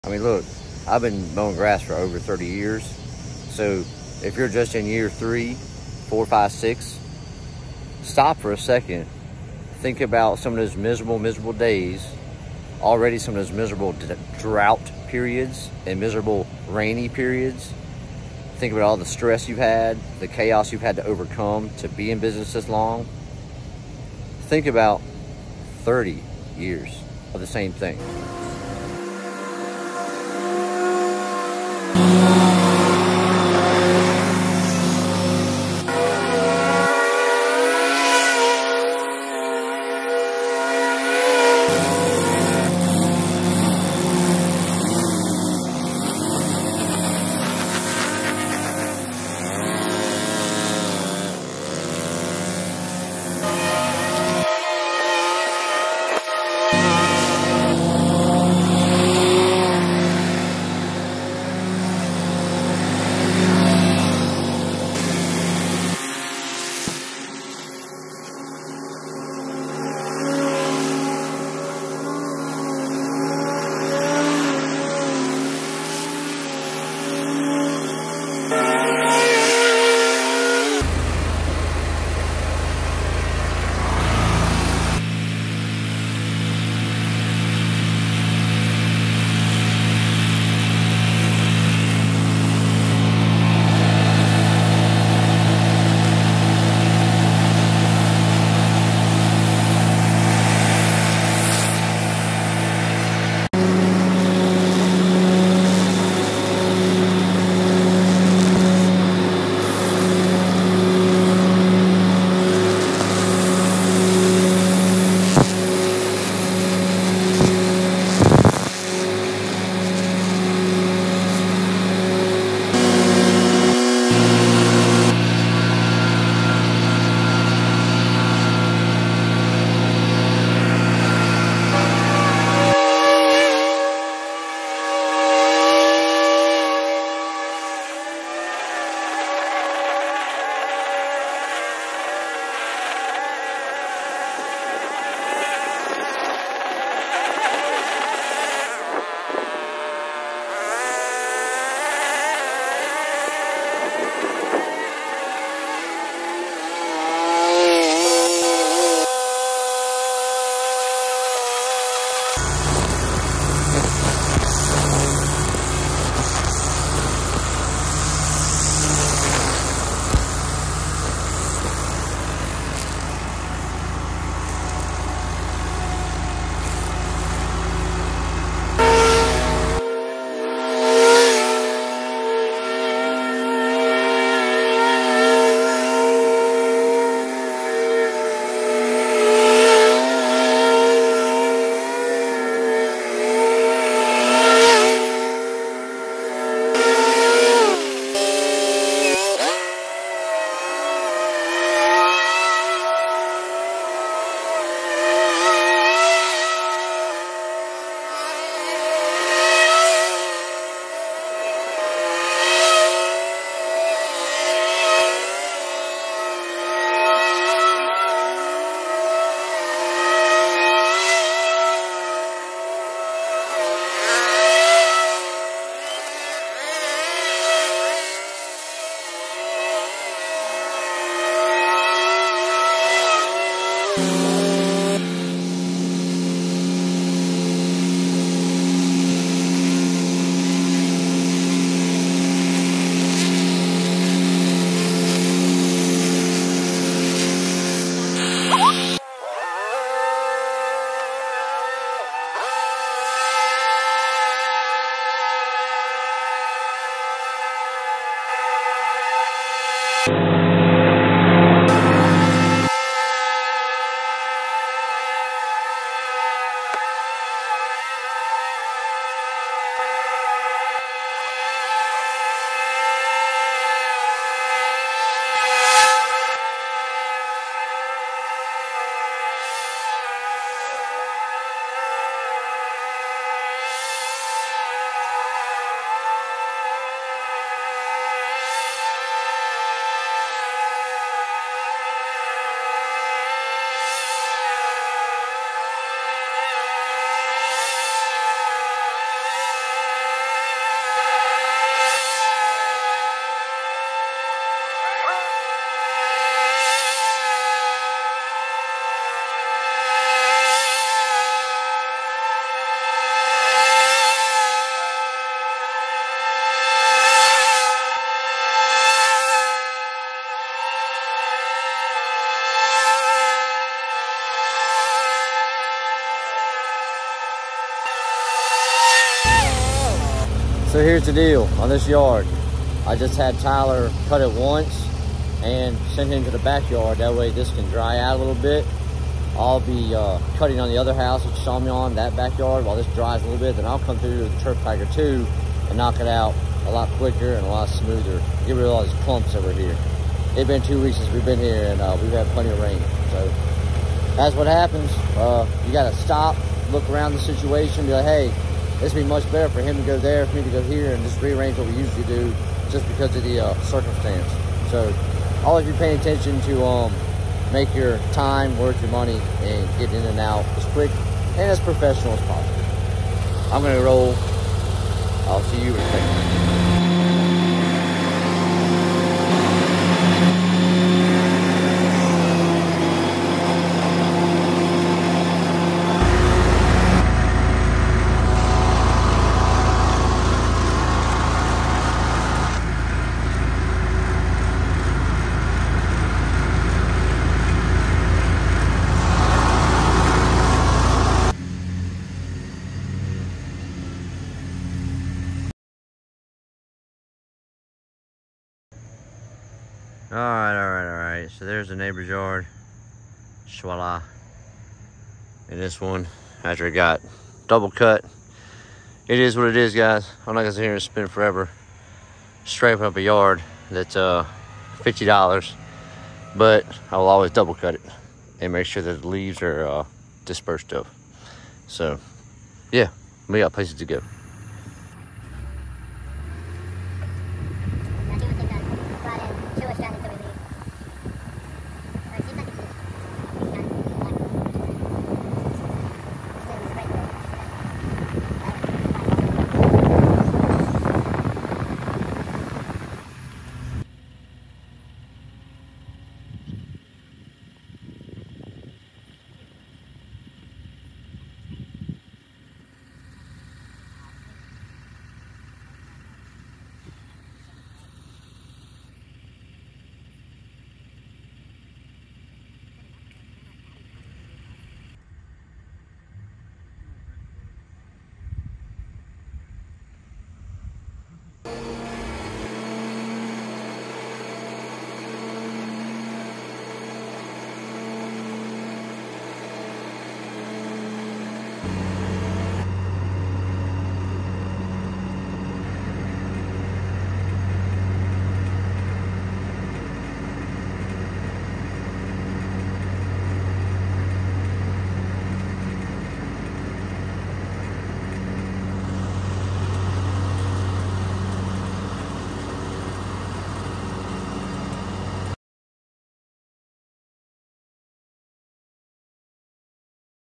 Mowing Grass… it “MUST BE PERFECT” thumbs up if YOU AGREE 👍
When you Mow Grass as a Profession, you clients expect you to be perfect in your work performance, your work ethics, and your communication with them. This is not an easy profession to be successful at. Mowing grass is so much more than JUST mowing grass. Watch how training your grass mowing crew to PERFECTION makes EVERY JOB EASY…. As a TEAM!!!